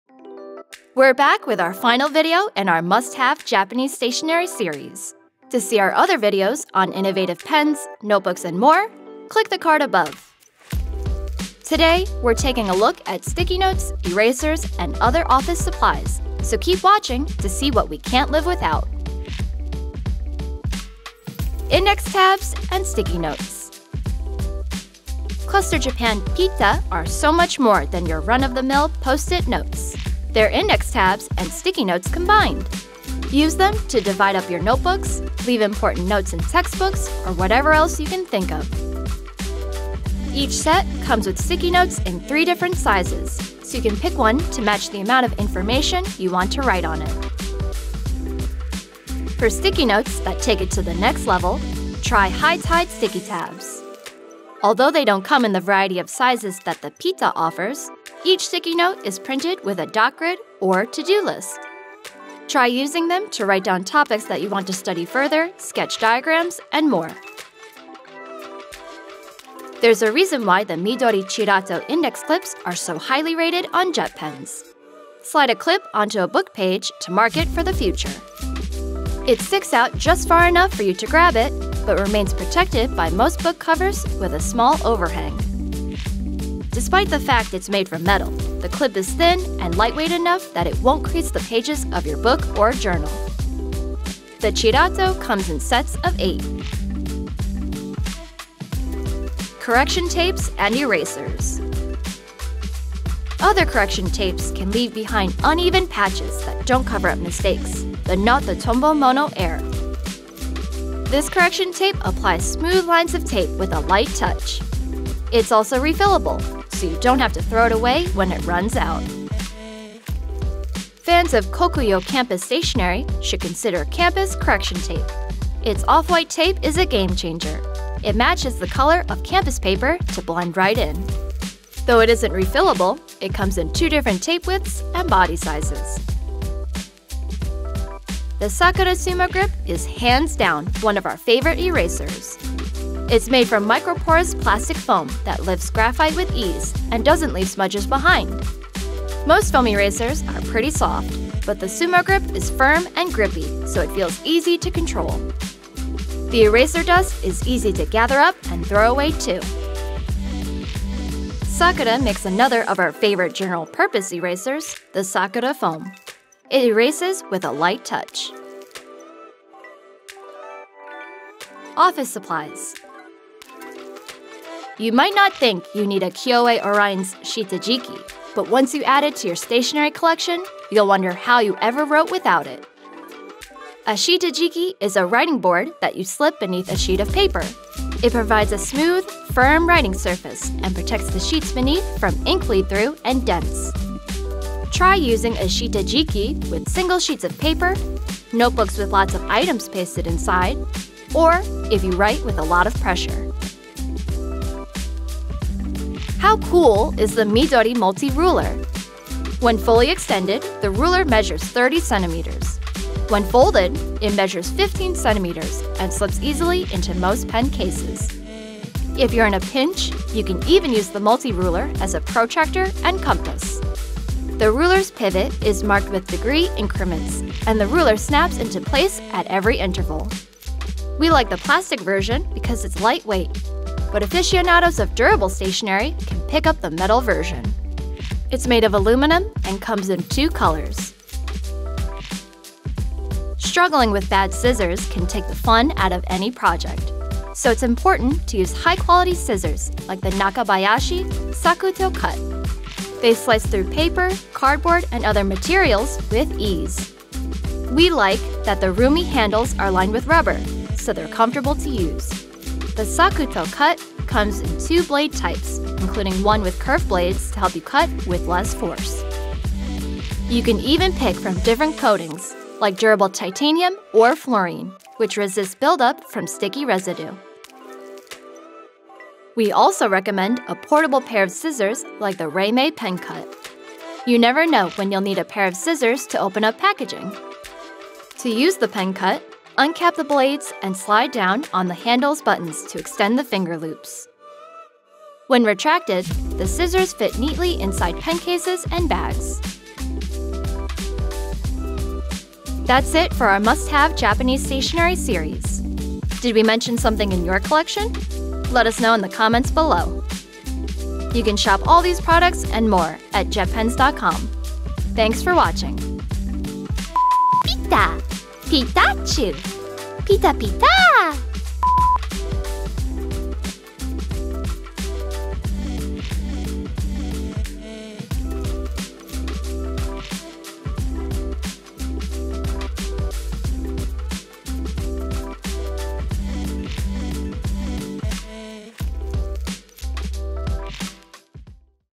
MUST-HAVE Japanese Office Supplies! ✨📎✂️ Innovative Sticky Notes, Rulers, & More!
In the last part of our Must-Have Japanese Stationery series, we highlight sticky notes, erasers, and other office supplies.

0:00 Intro
0:20 Index Tabs & Sticky Notes
1:38 Correction Tapes & Erasers
2:51 Office Supplies
5:01 Outro
5:17 Outtakes